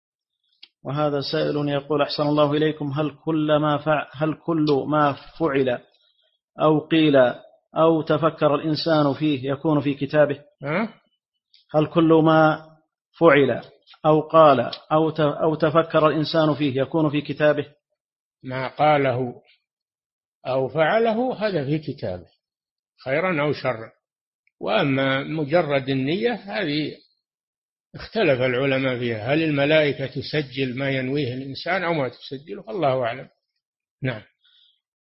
[209 -308] هل كل ما فعل أو قال أو تفكَّرَ الإنسان فيه يكون في كتابه؟ - الشيخ صالح الفوزان
-[209 -308] السؤال: هل كل ما فعل أو قال أو تفكَّرَ الإنسان فيه يكون في كتابه؟
الجواب: ما قاله أو فعله هذا في كتابه خيرًا أو شرًا، وأما مجرد النية هذه اختلف العلماء فيها، هَلْ الملائكة تُسجل ما ينويه الإنسان أو ما تُسجله؟ الله أعلم.
للاشتراك في قناة الشيخ صالح الفوزان

للاستفسار والإقتراحات والدعم
رئيس الفريق
لمشروع كبار العلماء